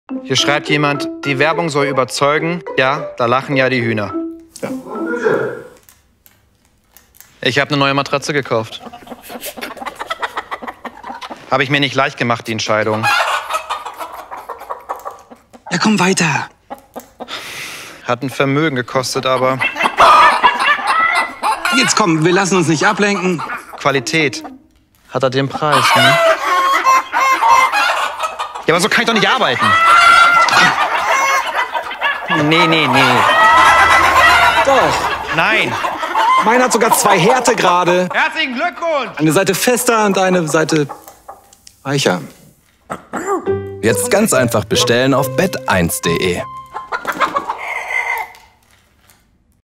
BETT1 Chicken
This was one of my first remote projects abroad. " Films for BETT1 , produced by Bubbles Film BErlin, directed by Caspar Jan Hogerzeil.
Would love to edit more like this. Children and Animals are always fun to go thru the footage.